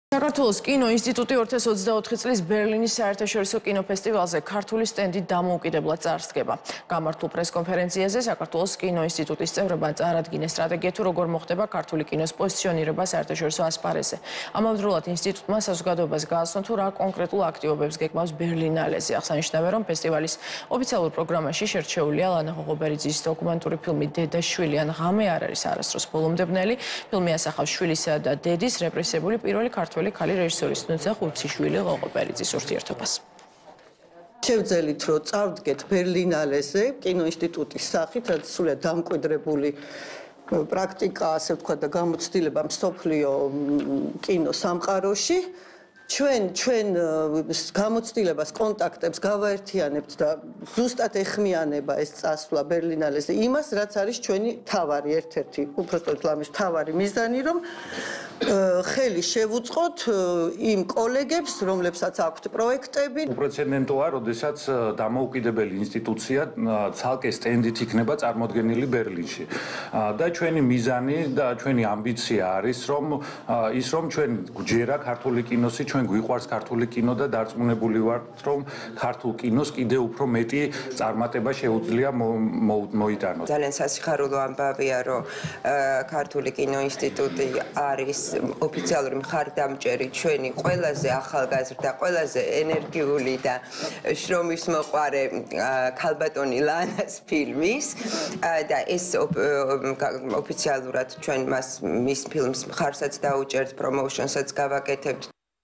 საქართველოს კინოინსტიტუტი ბერლინალეზე
საქართველოს კინოინსტიტუტი 2024 წლის ბერლინის საერთაშორისო კინოფესტივალზე ქართული სტენდით დამოუკიდებლად წარდგება. პრესკონფერენციაზე საქართველოს კინოინსტიტუტის წევრებმა წარადგინეს სტრატეგია, თუ როგორ მოხდება ქართული კინოს პოზიციონირება საერთაშორისო ასპარეზზე. ამავდროულად, ინსტიტუტმა საზოგადოებას გააცნო, თუ რა კონკრეტულ აქტივობებს გეგმავს ბერლინალეზე. აღსანიშნავია, რომ ფესტივალის ოფიციალურ პროგრამაში შერჩეულია ლანა ღოღობერიძის დოკუმენტური ფილმი „დედა-შვილი ან ღამე არ არის არასოდეს ბოლომდე ბნელი“. ფილმი ასახავს შვილისა და დედის, რეპრესირებული პირველი ქართველი ქალი რეჟისორის, ნუცა ხუციშვილი-ღოღობერიძის ურთიერთობას.